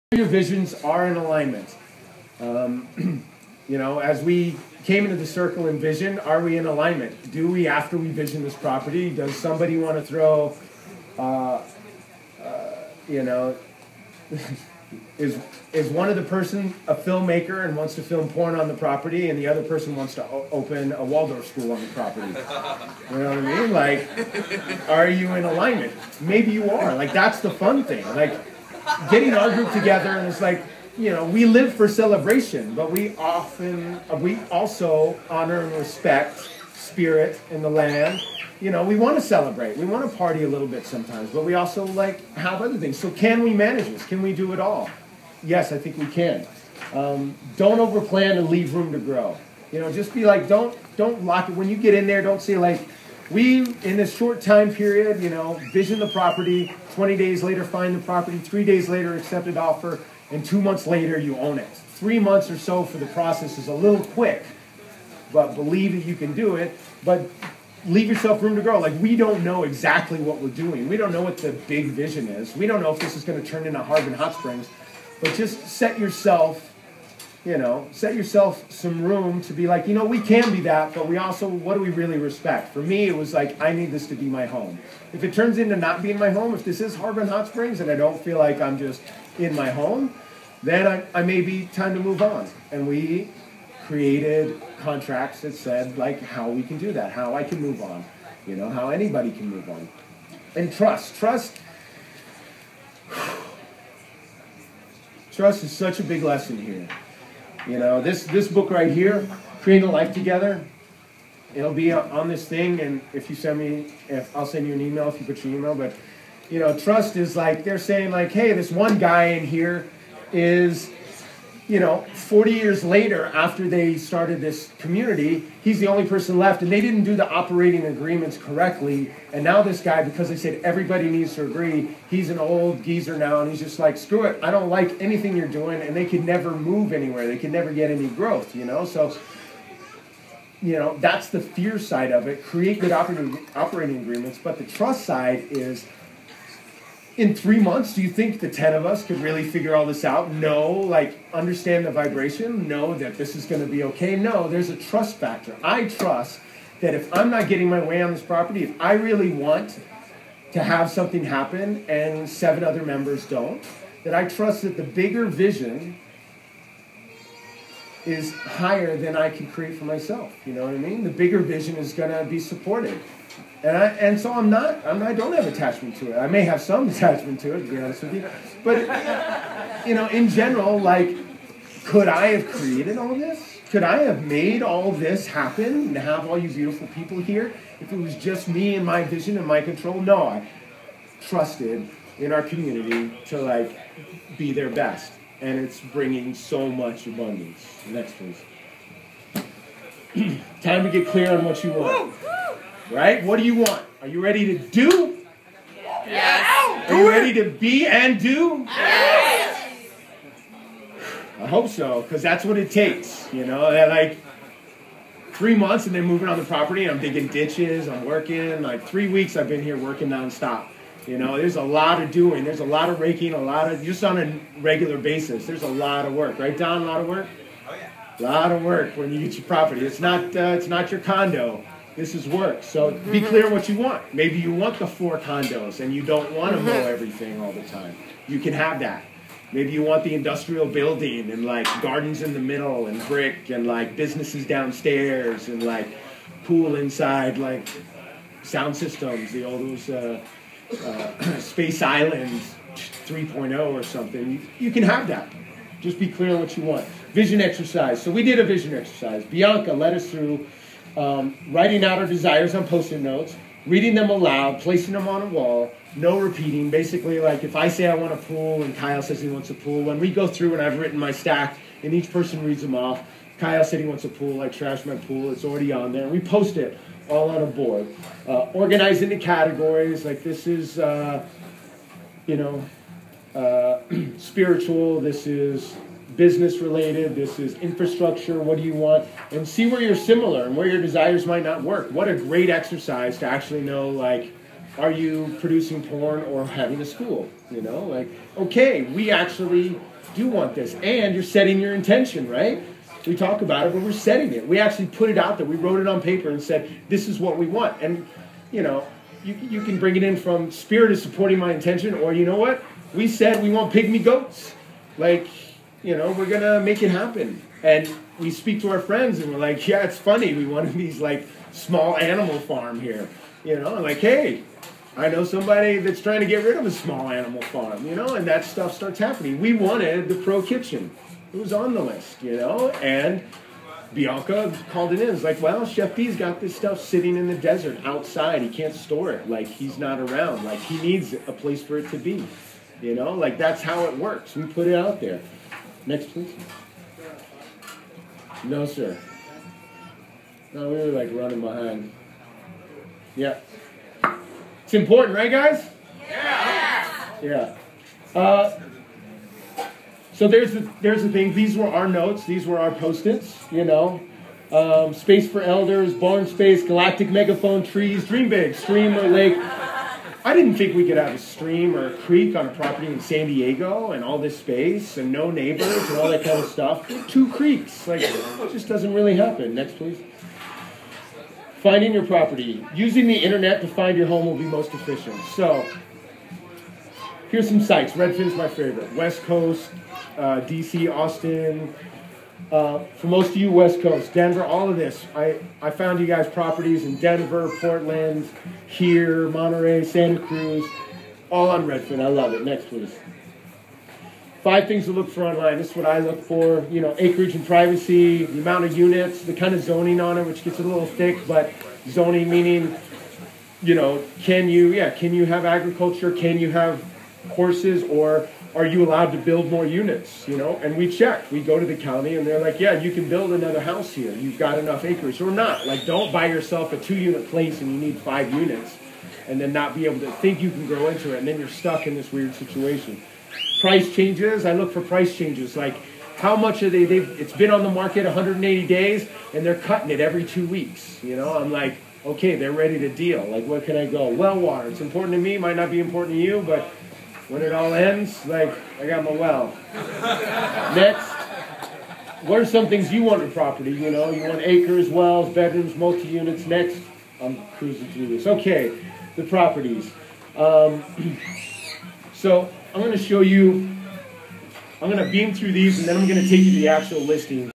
How to buy land as a community.. part 2: Aligning Community's Vision
Yohan of EVO, speaks at Tribal Convergence about STEP 1 to creating community.. getting clear with what you want and aligning each participants vision into an agreed upon community vision. More details on this process coming soon!